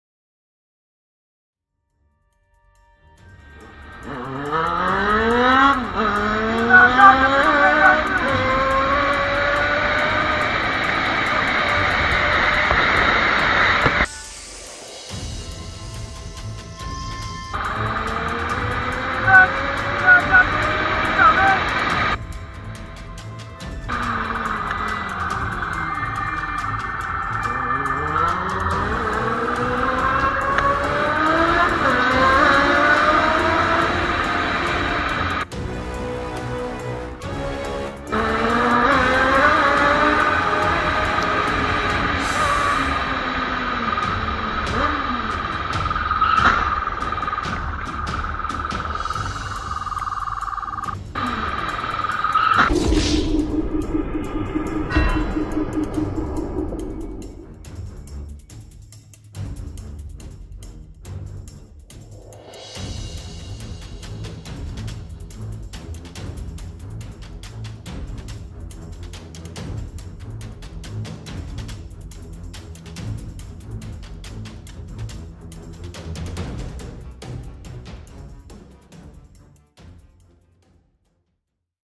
Motorcycle Police Chase Through Brazilian City Streets
Watch till the end to see what happens.

If you like the videos on this channel, please help out by making a donation to help keep the lights on.